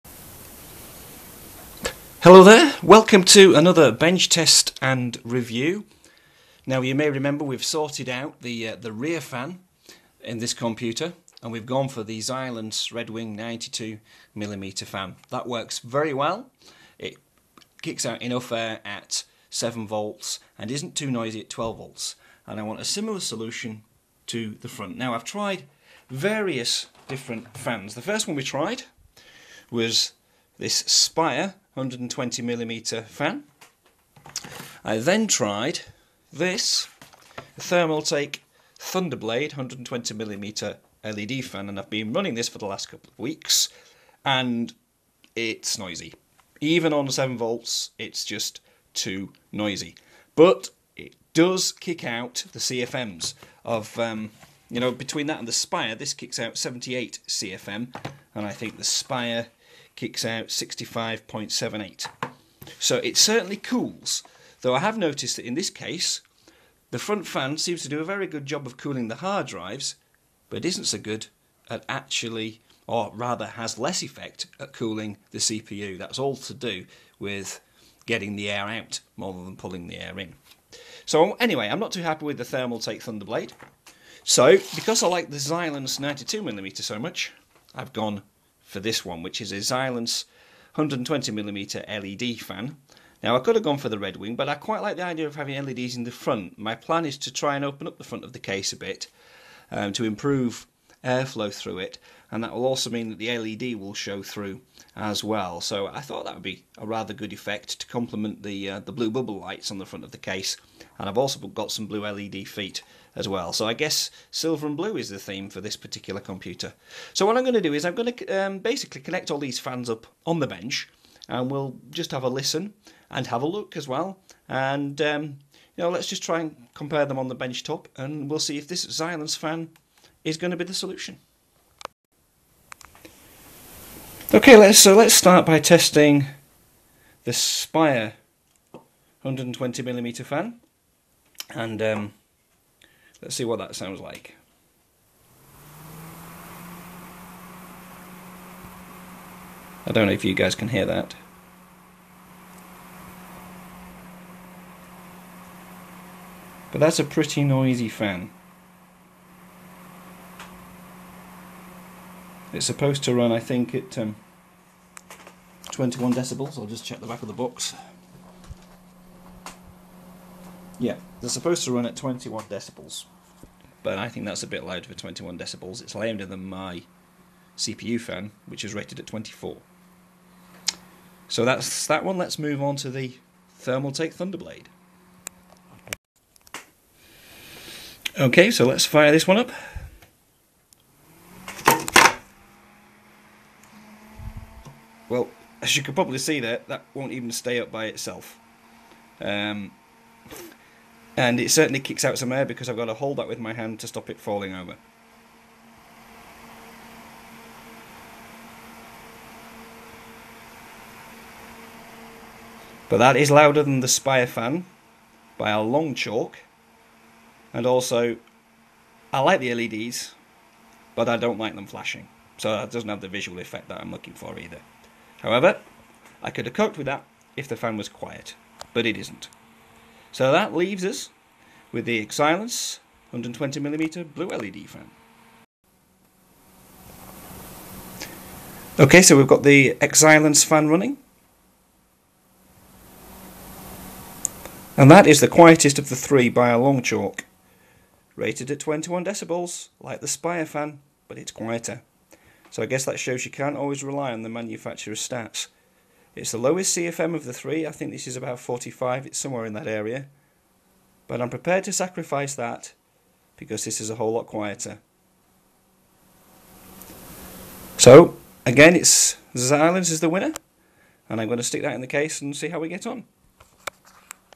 Xilence 120mm Blue LED Fan - Benchtest and Review
Another old video I made and a follow up to the 92mm fan benchtest. This is a great fan which cools well and is not too loud.